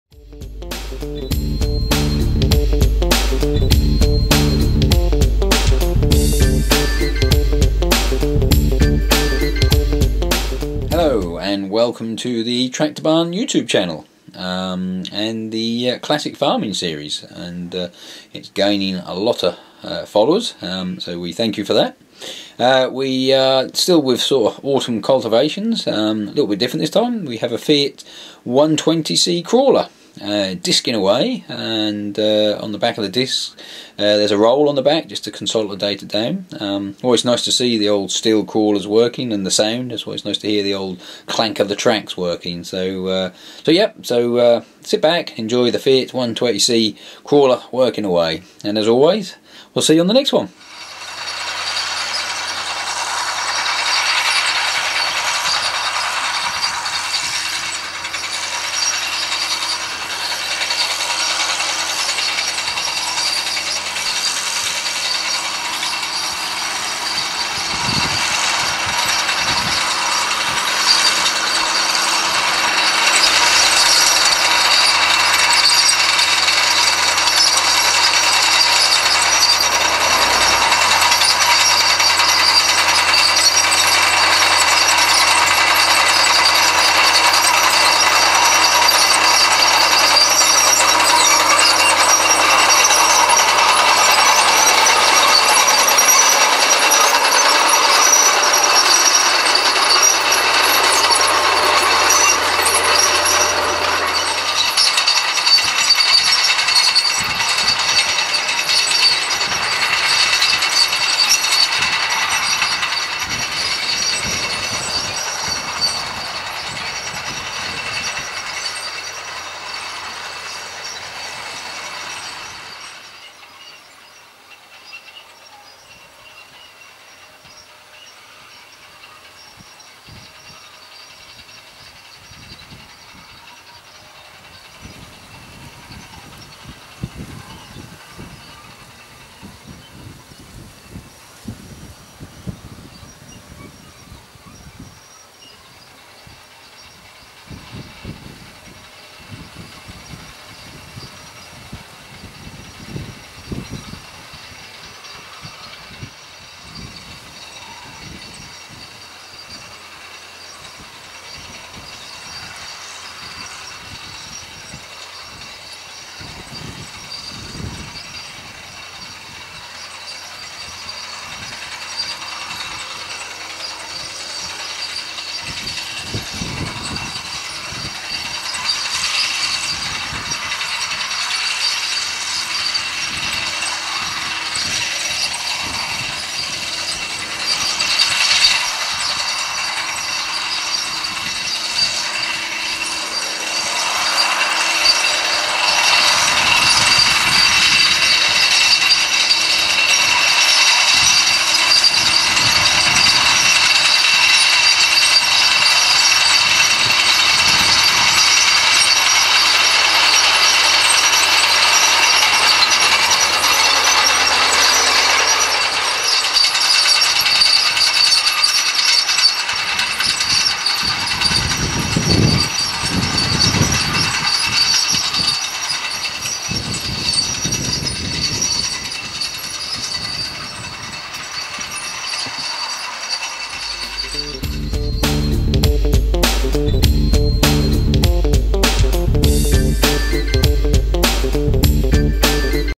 FIAT 120C CRAWLER AND DISC HARROWS
CLASSIC FARMING 58: This weeks video is from Suffolk in the autumn of 2000 and features a Fiat 120C crawler using a set of disc harrows with a roll behind.